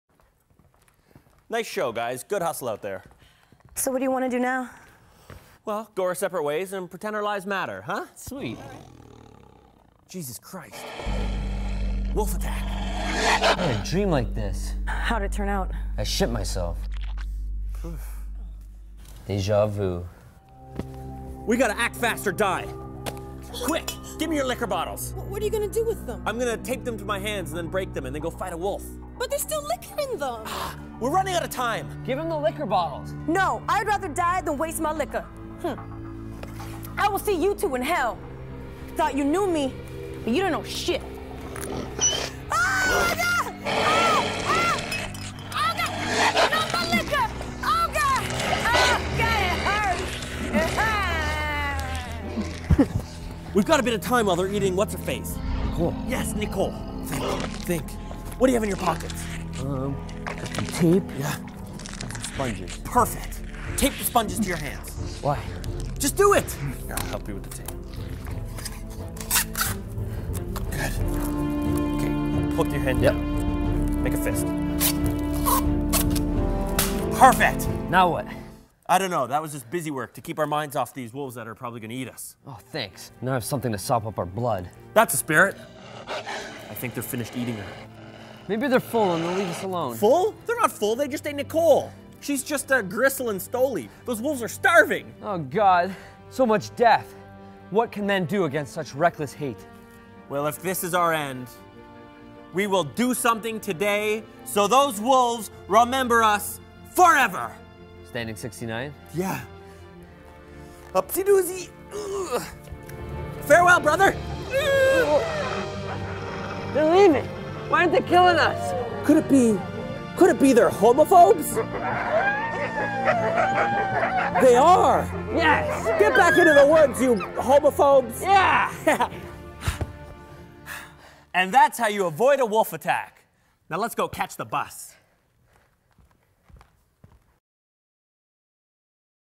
Work Wolf Attack
TheDarynJones. Daryn, Nicole, and Paul attempt to leave after a day's work, but find there's something in their way... wolves. Daryn Jones